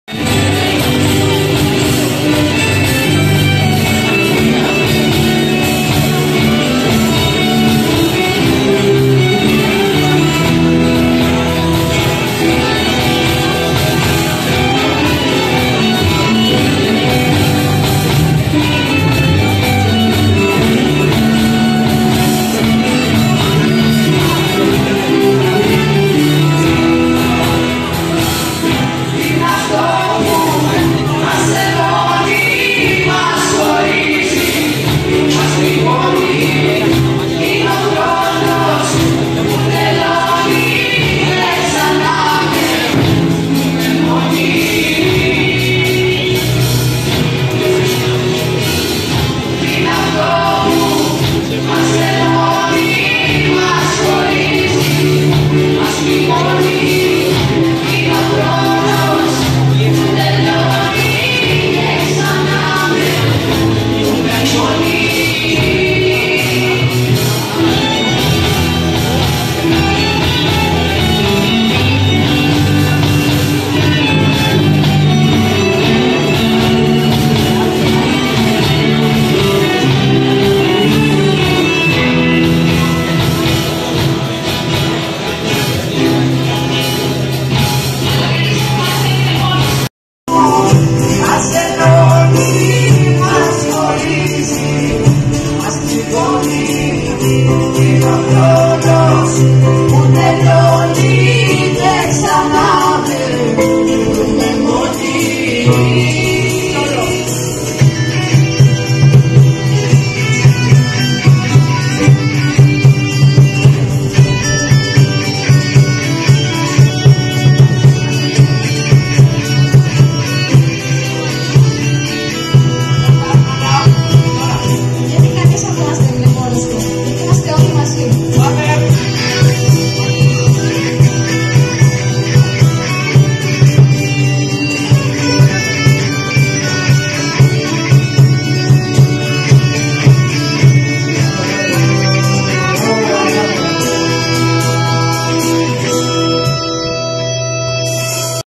Γιορτή πολυτεχνείου 2022 "Τι είναι αυτό που μας ενώνει"
Μια από τις μεγαλύτερες σε διάρκεια και συμμετοχή γιορτή για την επέτειο της εξέγερσης του Πολυτεχνείου με συμμετοχή 1ου και 2ου ΓΕΛ Κερκύρας.